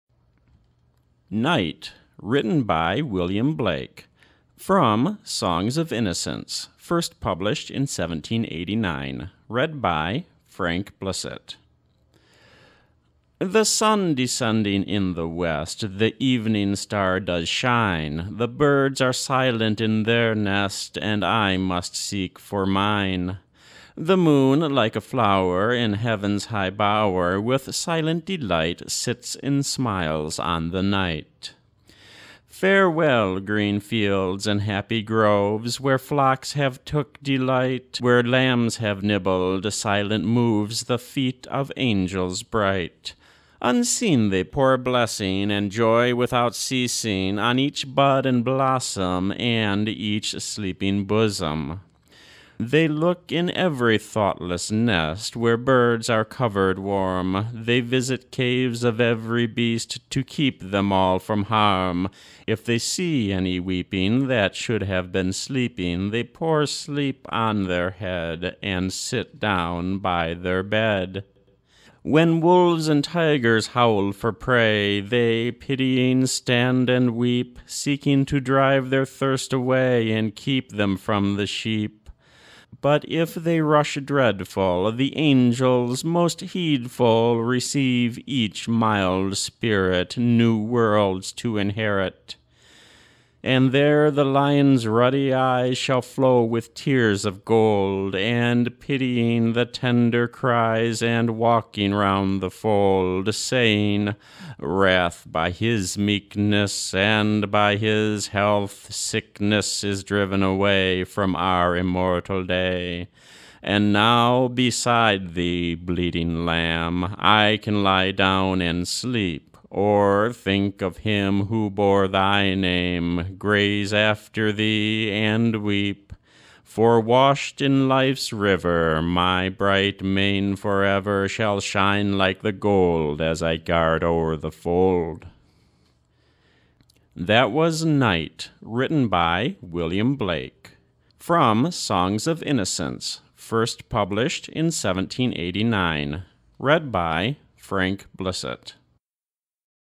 Night, from 'Songs Of Innocence', by William Blake
Night, from "Songs Of Innocence" (1789).

Written by - William Blake
Read by - Frank Blissett